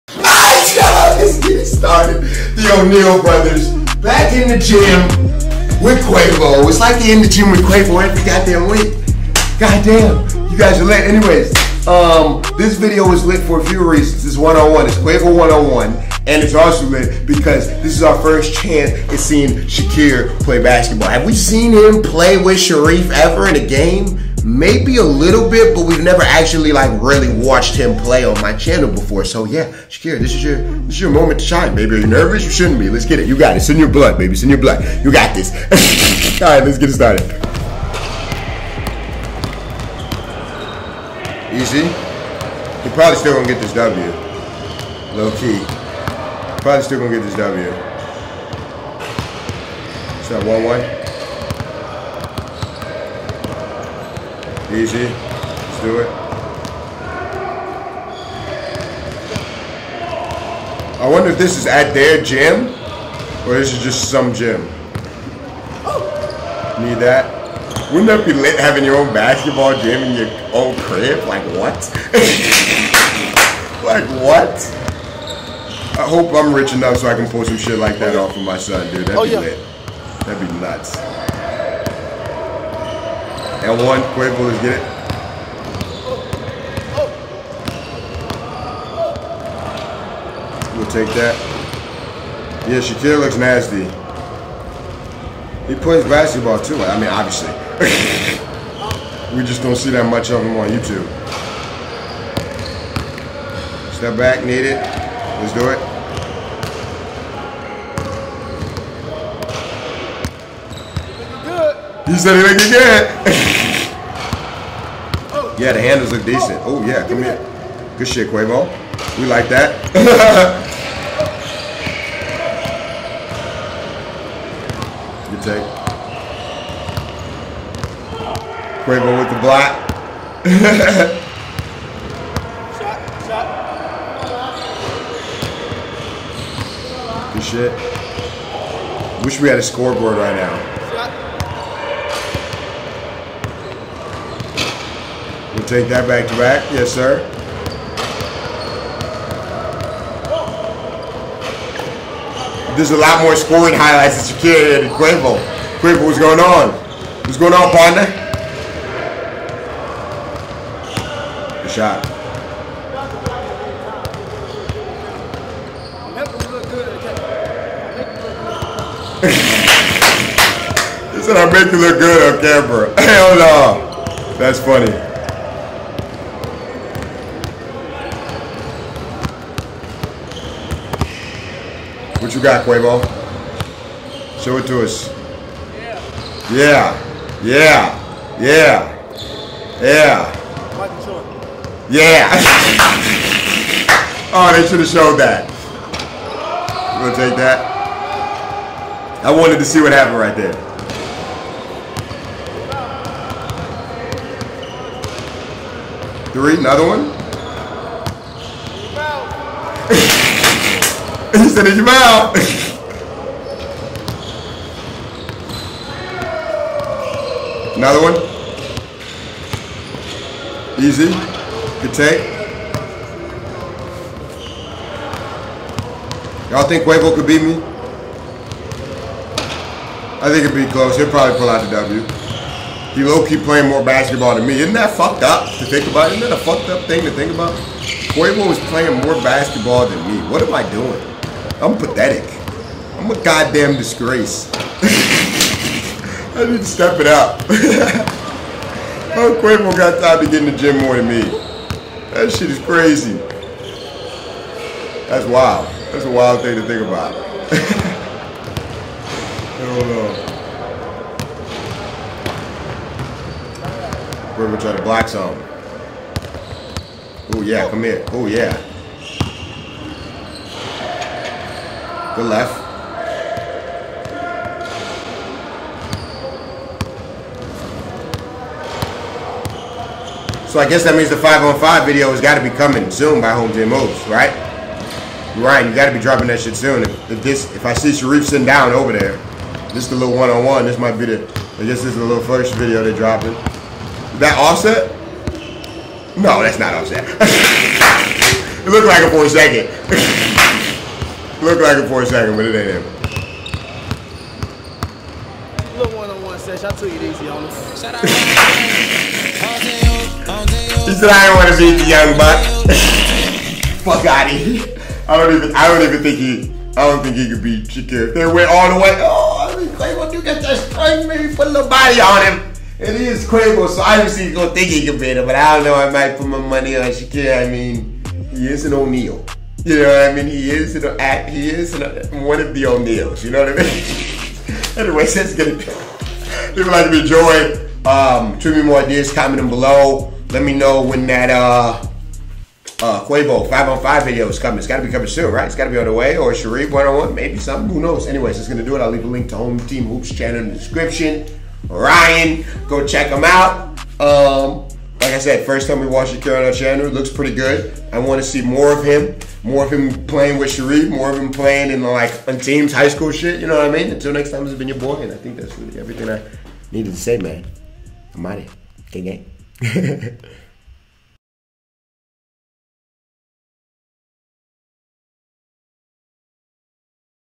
QUAVO THINKS HE NICE LOL! SHAQIR O'NEAL vs QUAVO 1 on 1 REACTION!
QUAVO GOING UP AGAINST SHAREEF'S BROTHER SHAQIR IN A GAME OF 1 ON 1!